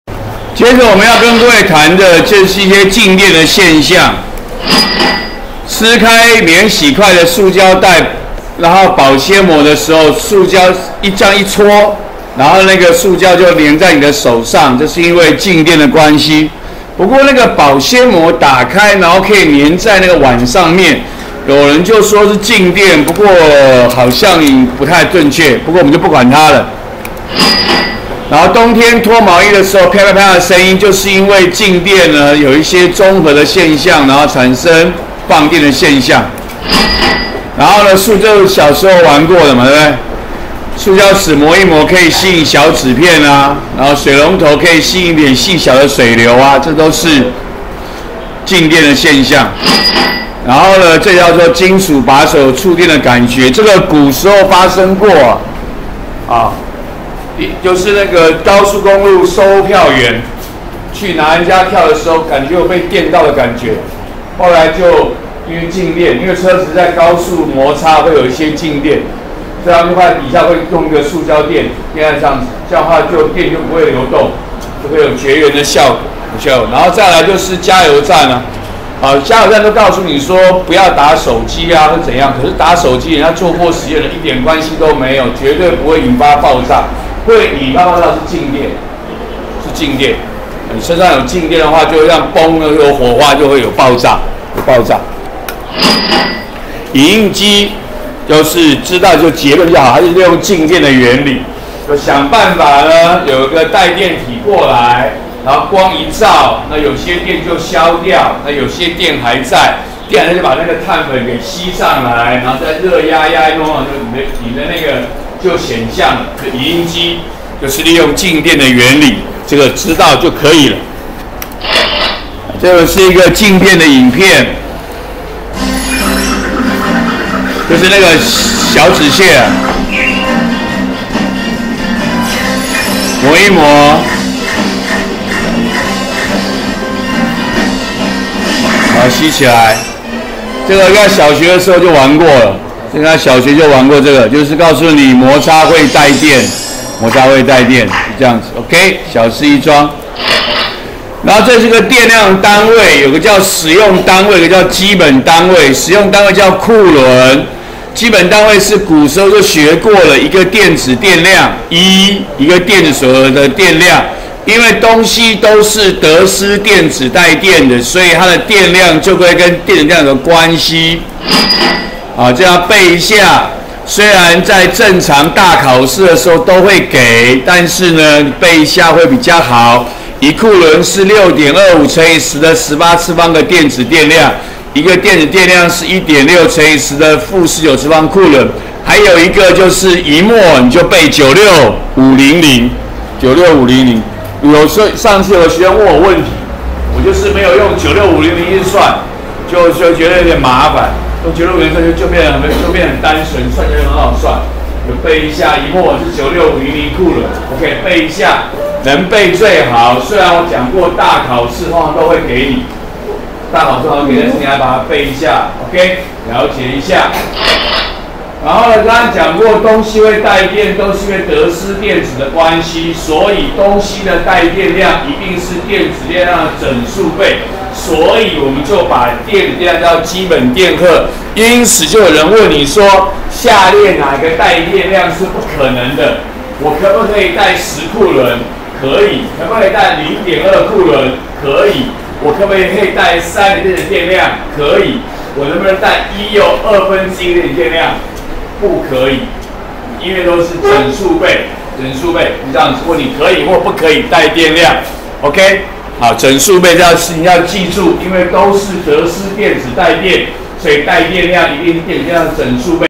104學年國三理化＿靜電03：靜電現象；靜電單位與關係
介紹因靜電作用產生的現象。播放靜電影片。介絡電量的單位，及不同單位的換算，及帶電量與電子電量的關係。